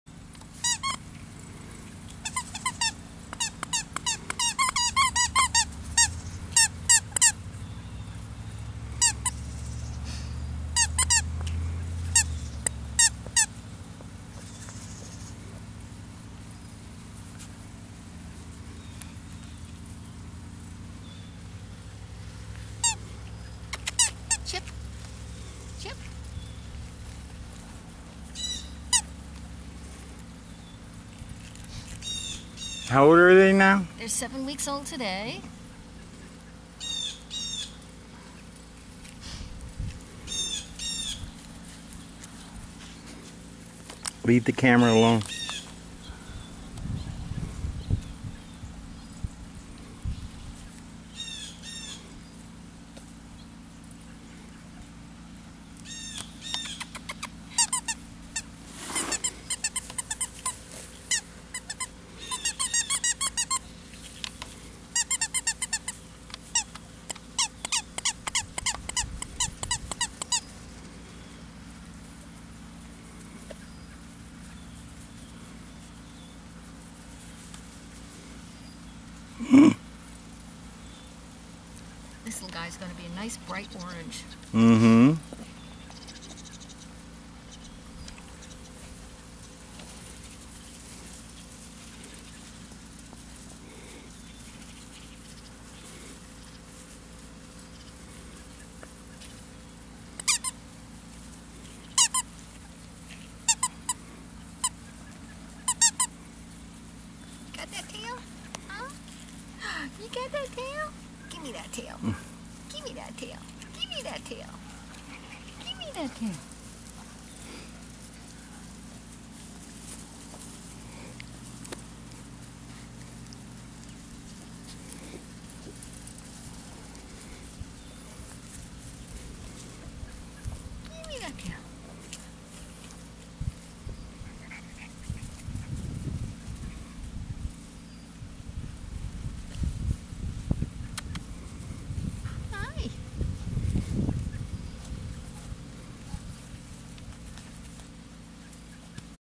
Bazinga's 7 week old puppies, video 2
Cherie, Chip, Charger and Chevas love playing outside, but their favorite "playthings" are people.